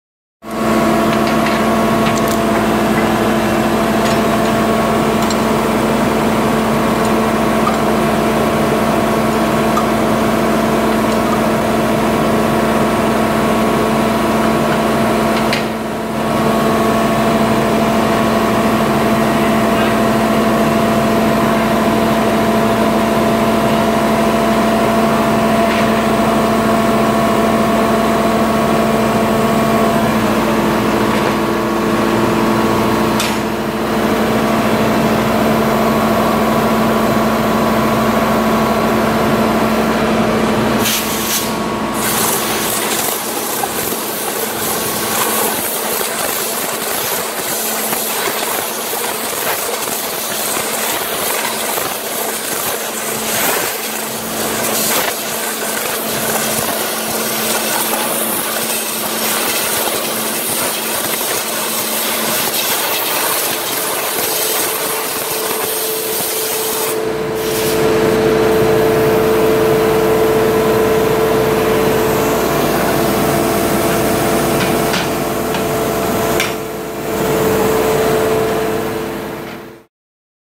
Making Rubber Mold for Pencils - Southern Michigan Rubber
Southern Michigan Rubber - Three Rivers, MI